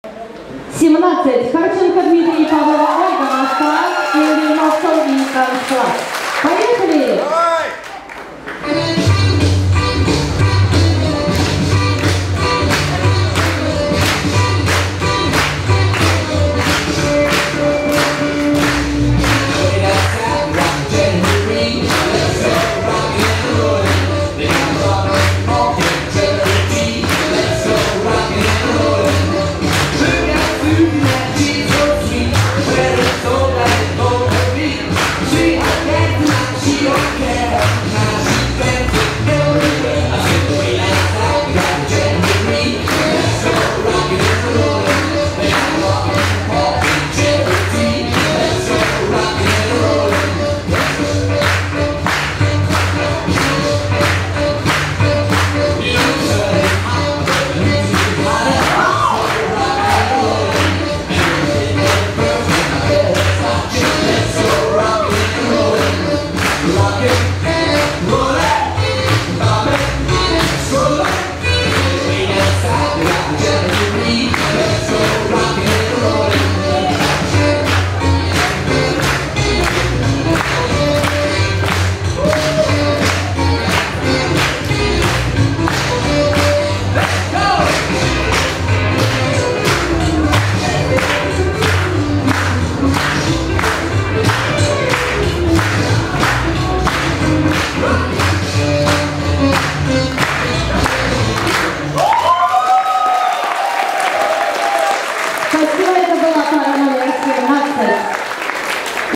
Харченко Дмитрий & Павлова Ольга - Boogie Woogie C-Class Finals at Sultans of Swing 2014
Sultans of Swing, Boogie-Woogie, Moscow, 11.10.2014